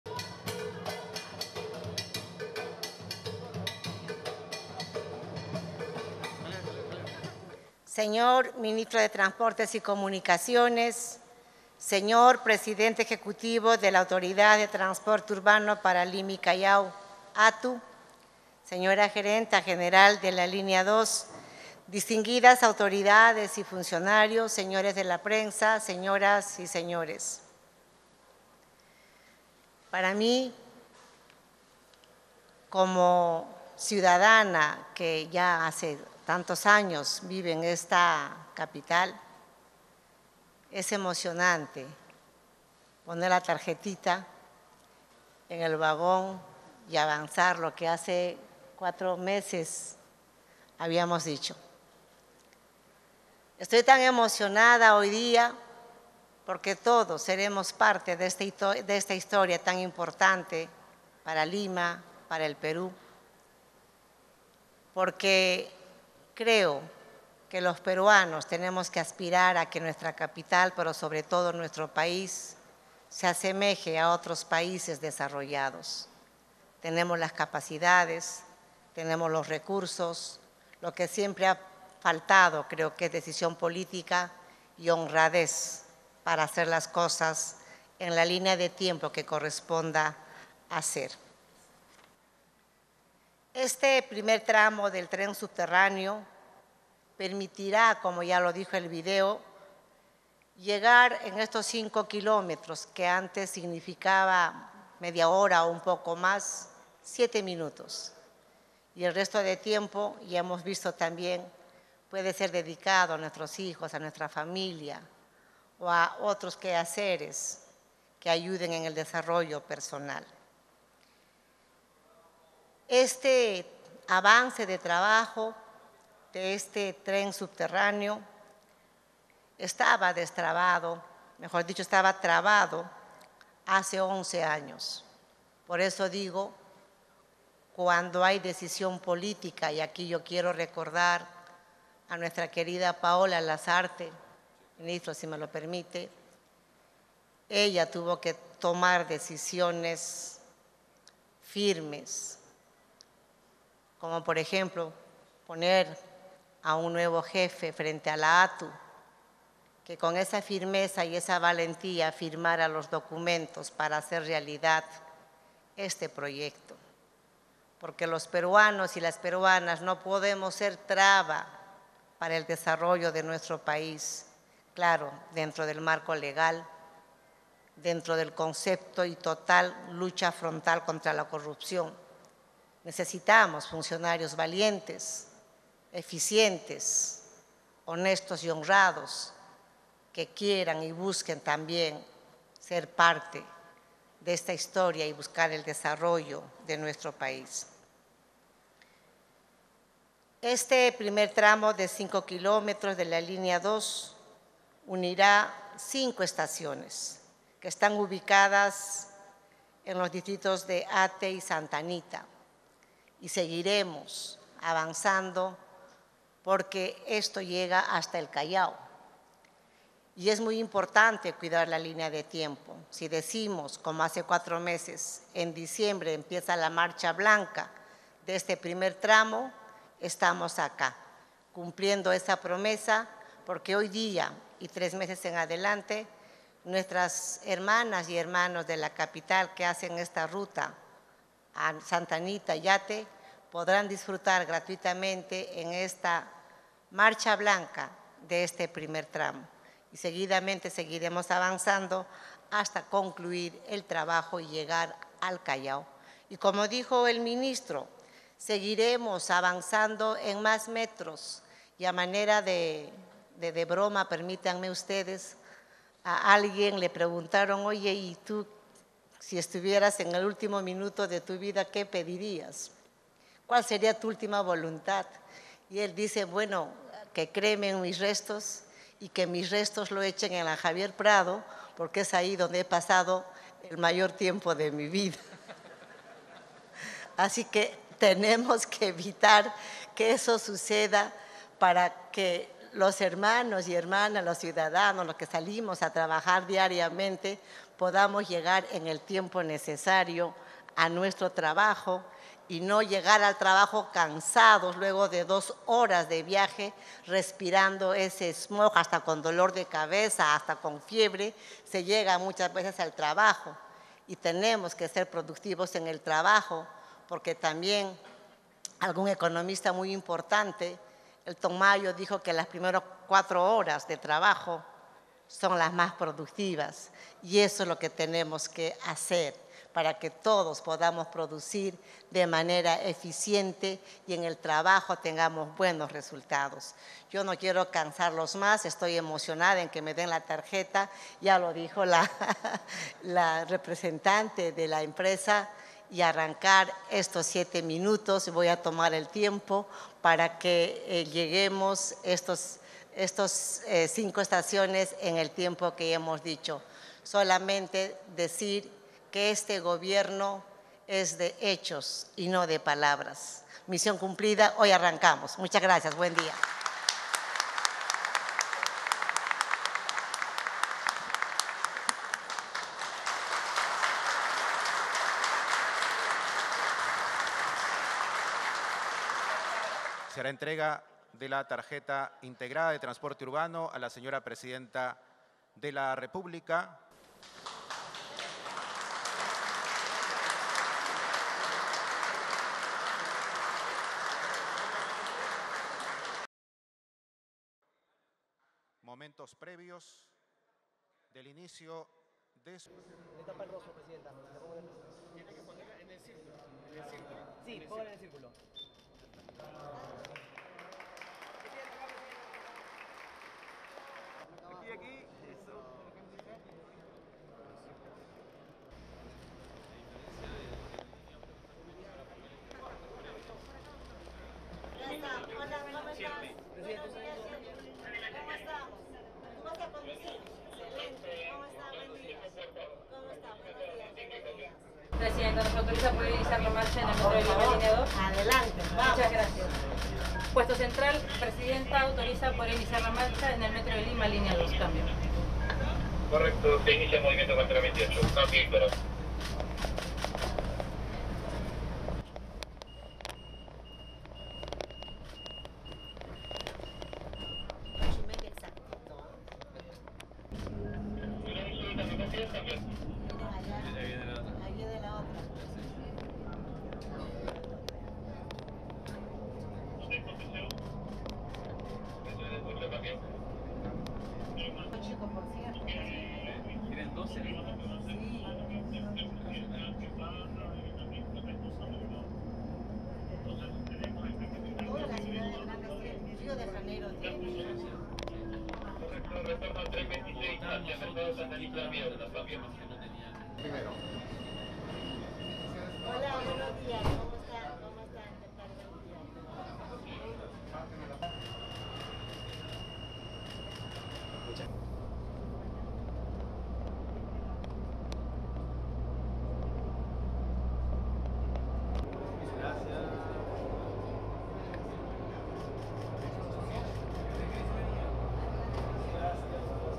Supervisión de operaciones de la etapa 1A de la Línea 2 del primer metro subterráneo Lima y Callao
La presidenta Dina Boluarte supervisa el inicio de operaciones de la etapa 1A de la Línea 2 del primer metro subterráneo Lima y Callao